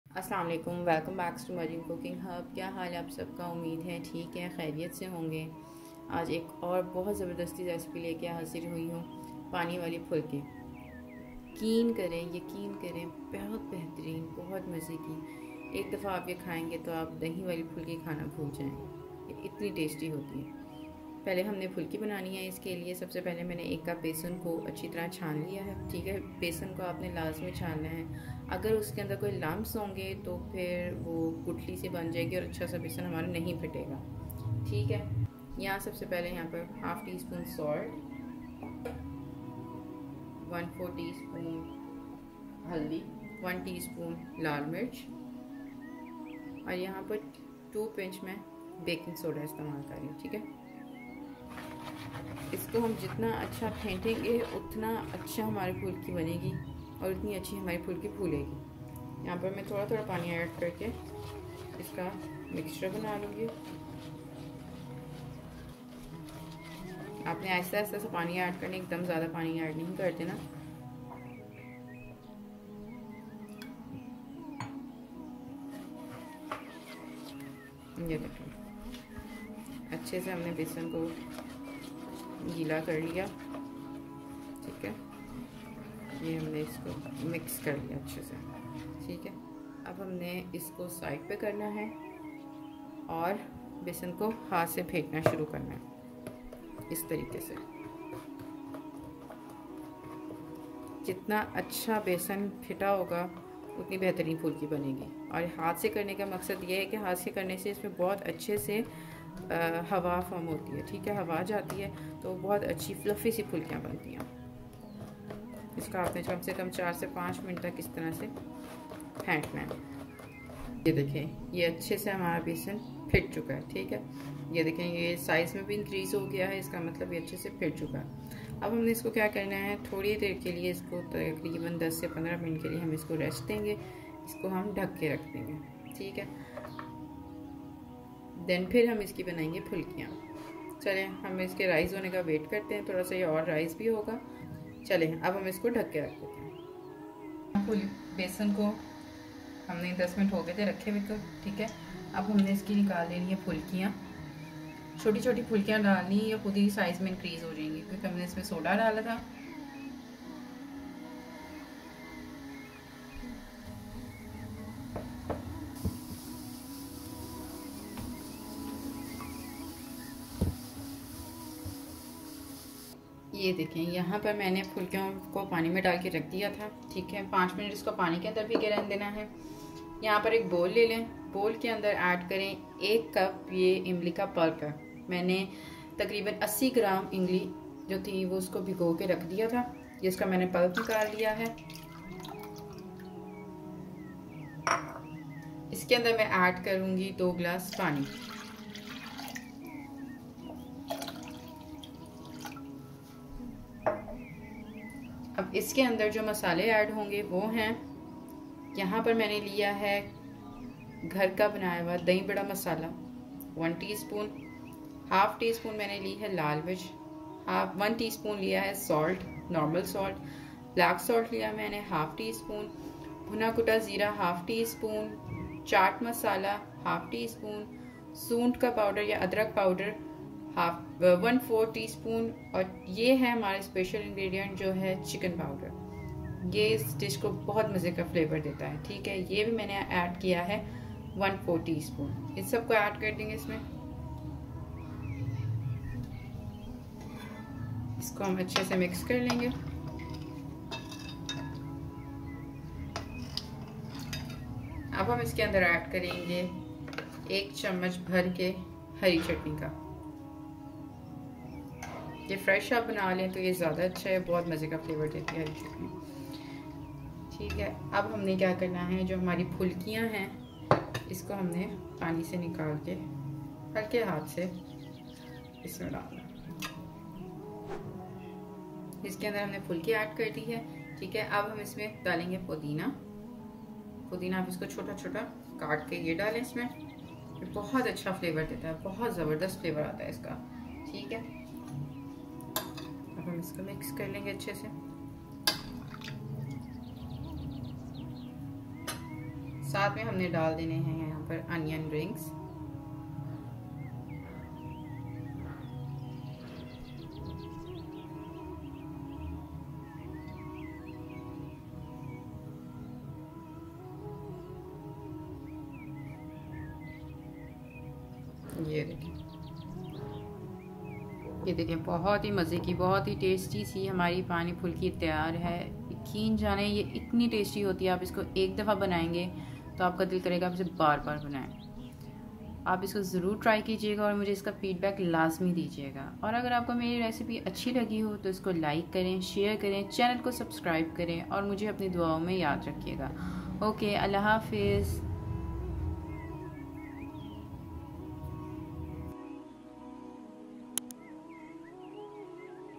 Pani Phulki Ramadan Special Recipe 🌙 | Maryam's Cooking Hub
Ingredients:
Lal Mirch Powder - 1 Tsp
Baking Soda - 2 Pinch
Tamarind Pulp - 1 Cup
Tamarind - 80 Gram
Water - 2 Glass
Dahi Bahry  Masala - 1 Tsp
Lal Mirch Powder - 1/2 Tsp

Green Chutney - 1 Tbsp
Mint
Onion Rings